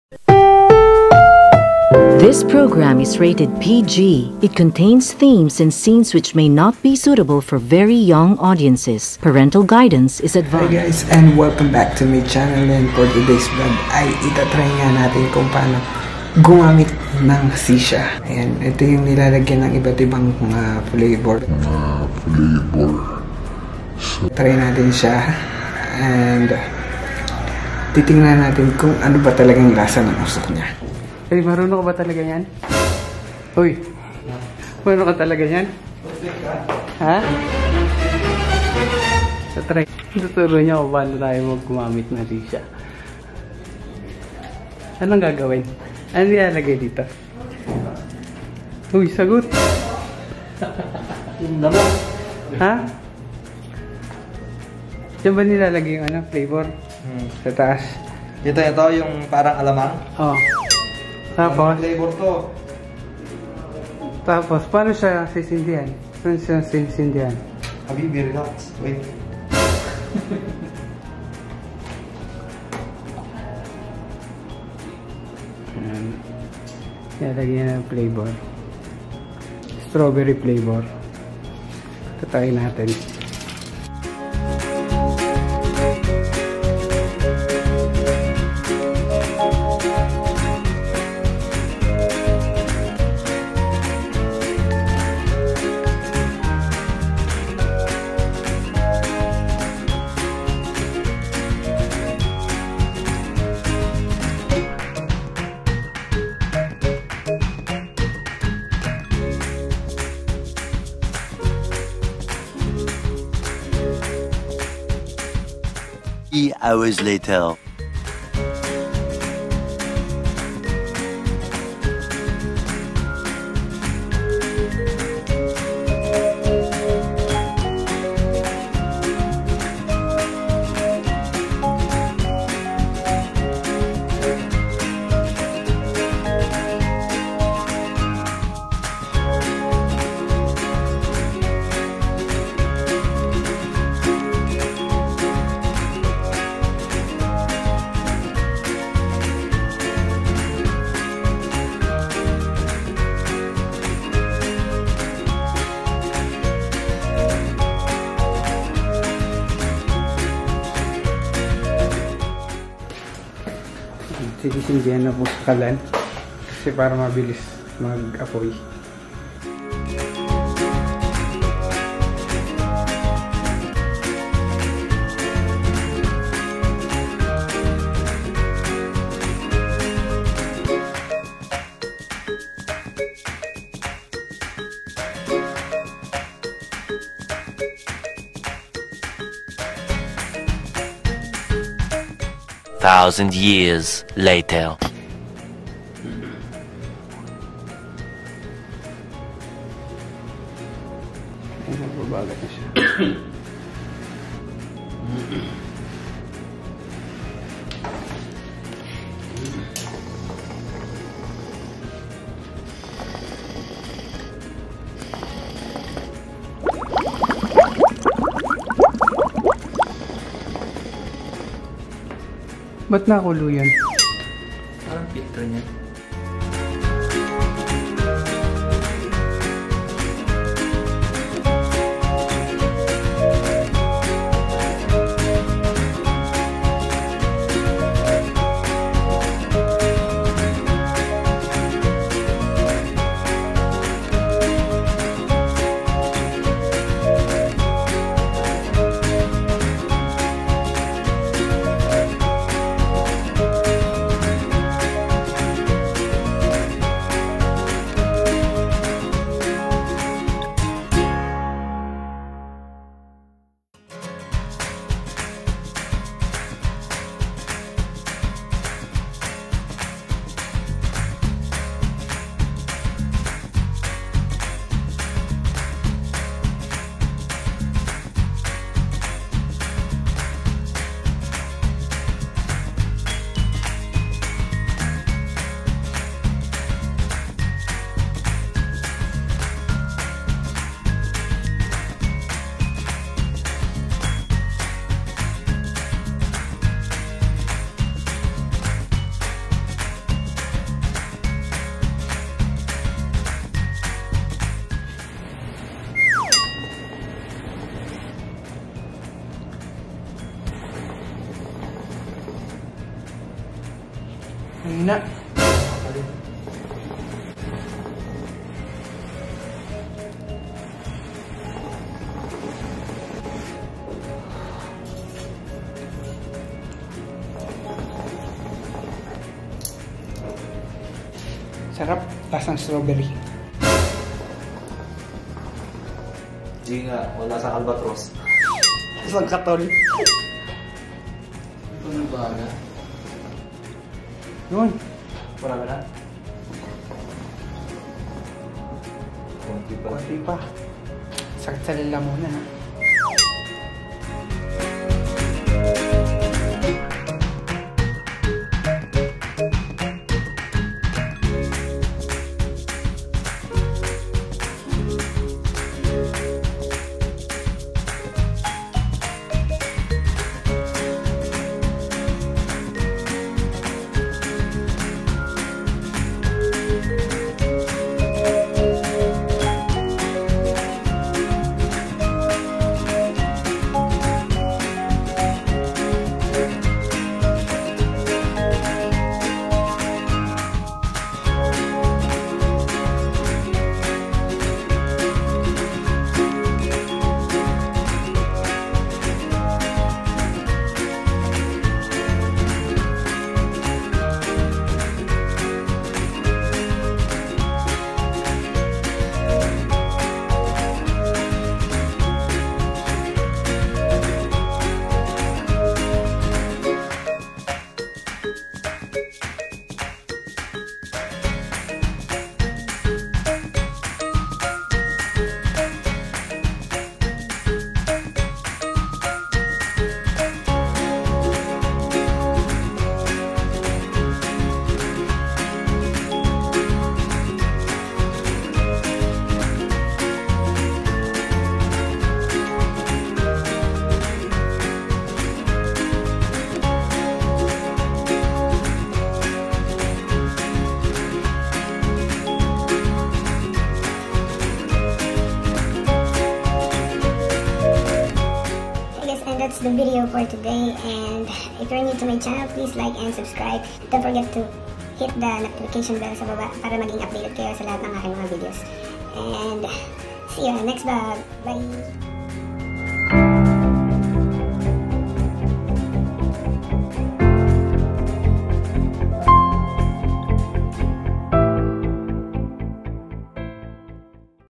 SHISHA VLOG | SUMAKIT LALAMUNAN KO ( LASANG ALBATROSS AT KATOL )
Guys remember CIGARETTES SMOKING IS DANGEROUS TO YOUR HEALTH.. thank you po sa lahat ng sumusuporta skin.. iloveyou guys